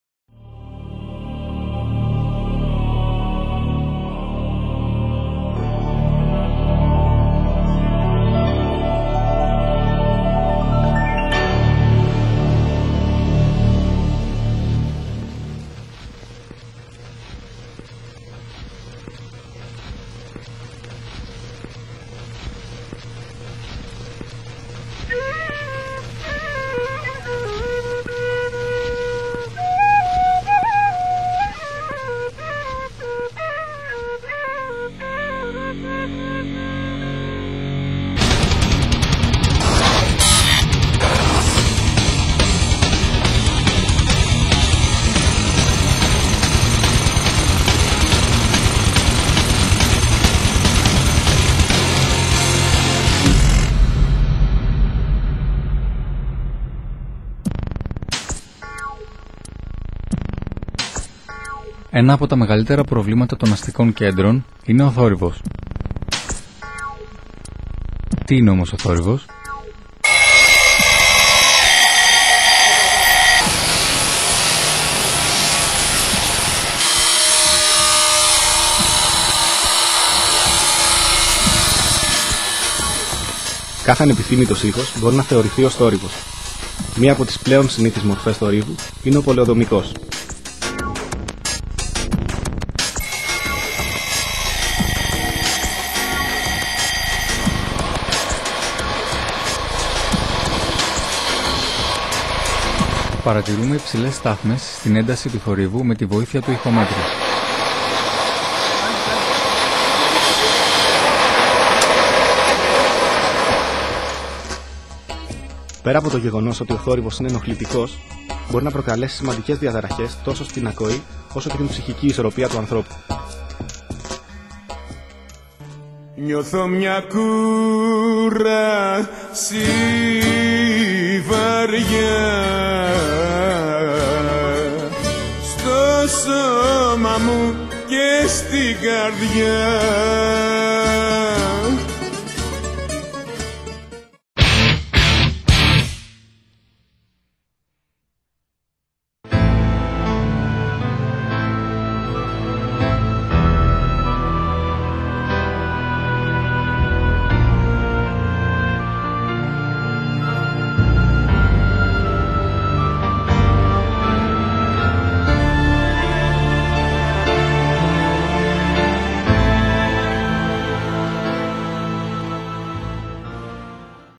Γαμάτη Ομάδα 2
(Kind of) A short documentary film about Noises In Cities made by my colleagues and me for a class project in University.